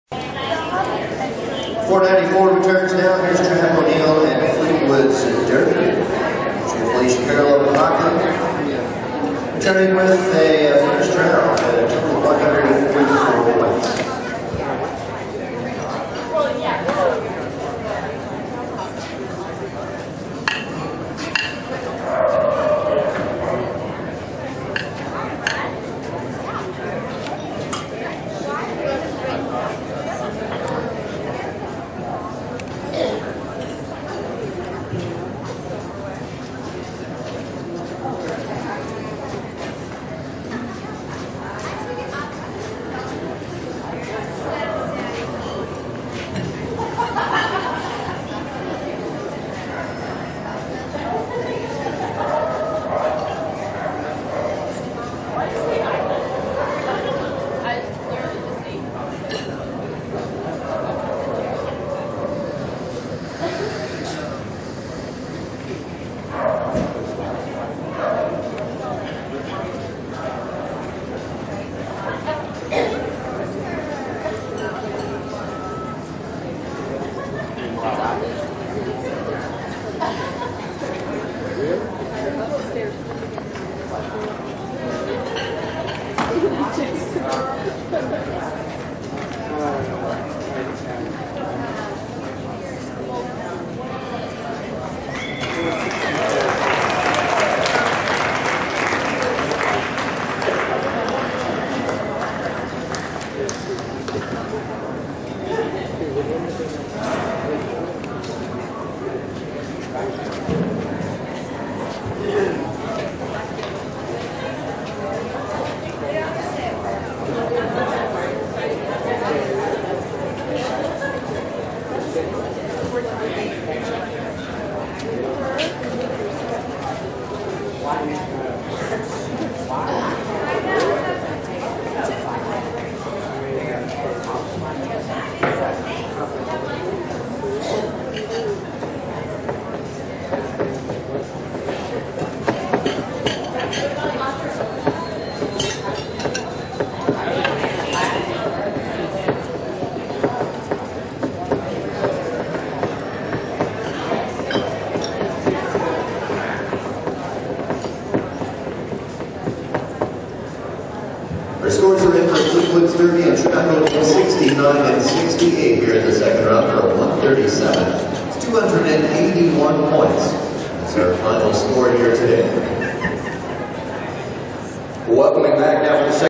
Video of FLEETWOOD'S DERBY ridden by TRAPP O'NEAL from ShowNet!
FLEETWOOD'S DERBY ridden by TRAPP O'NEAL at Final Chase 2013.  This round is from class 306, TX pre-Green Super Stake Final and finished 19 of 31.